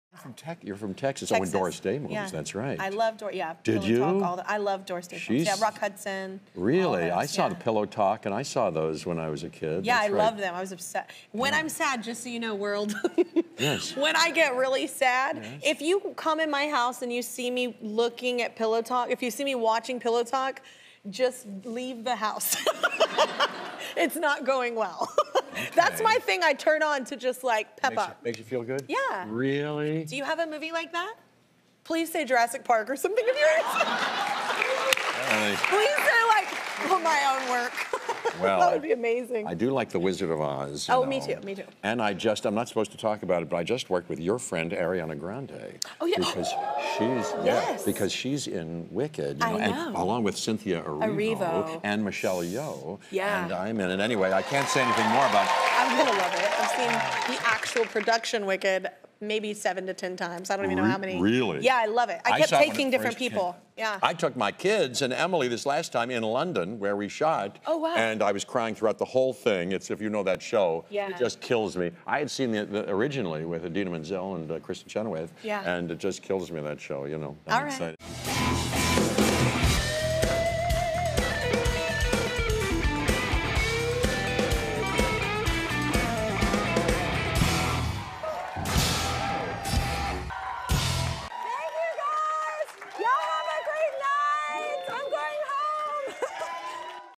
Jeff Goldblum Teases Ariana Grande 'Wicked' Movie Musical
"I'm not supposed to talk about it..." During a conversation about their favorite music, Jeff Goldblum can't help but mention his excitement about working with Ariana Grande, Michelle Yeoh and Cynthia Erivo in the upcoming "Wicked" movie musical! Tune in today for an extra special performance with Jeff Goldblum and the Mildred Snitzer Orchestra.